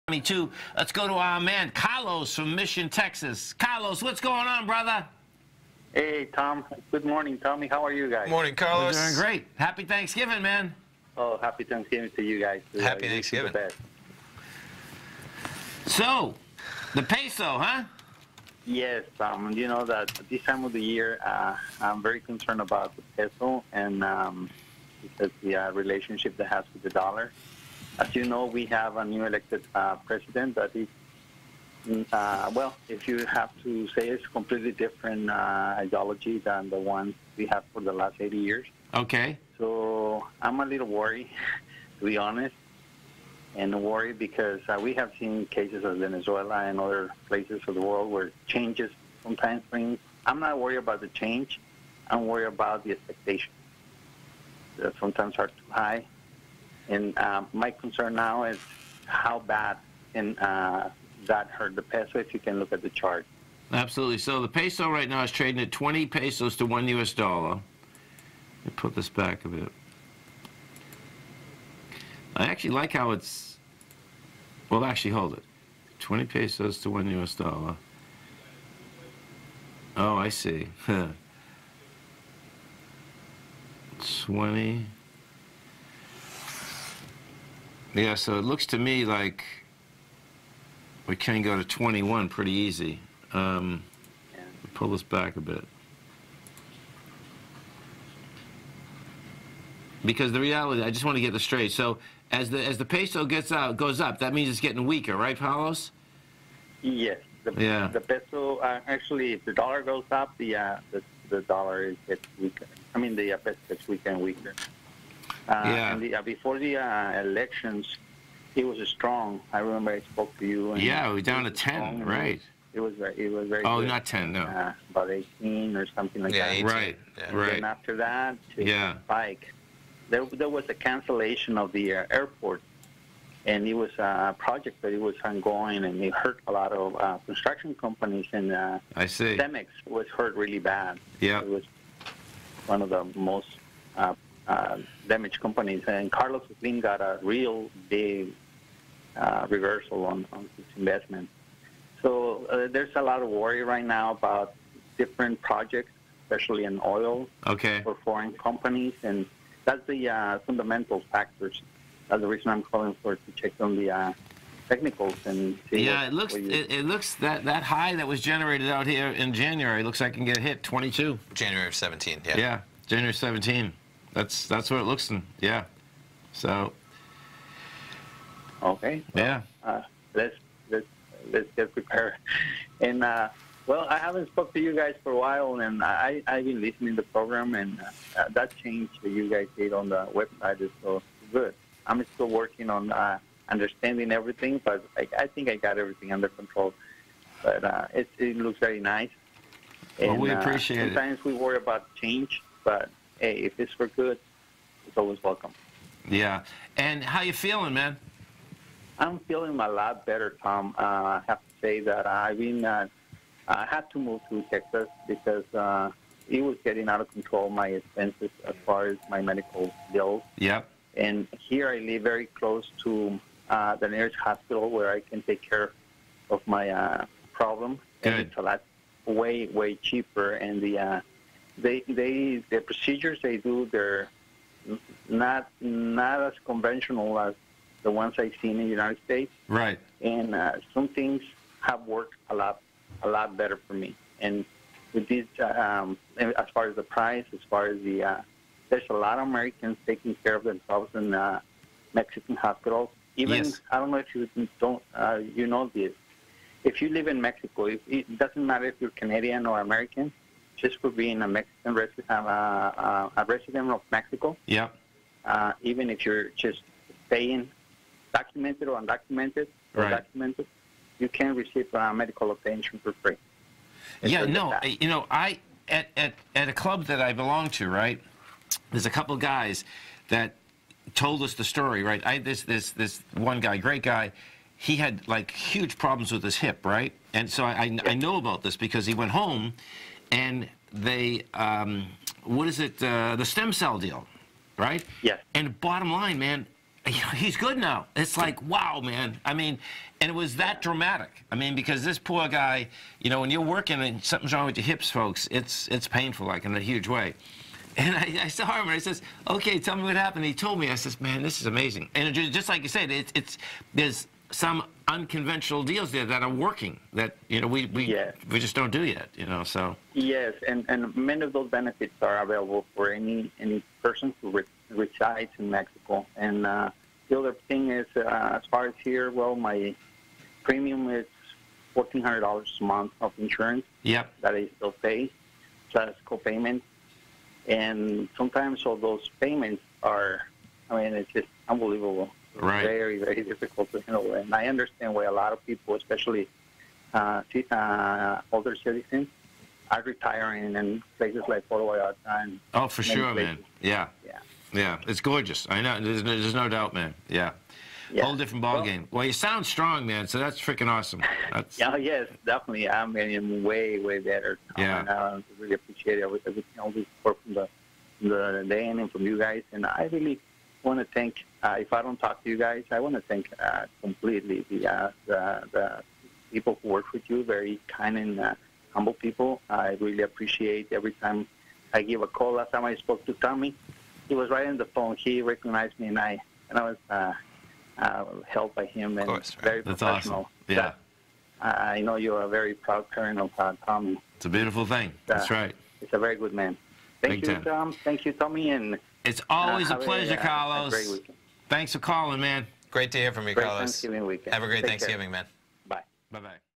What Does The Future Of The Peso Hold?
Caller from Texas wants to talk the Peso and the state of its future.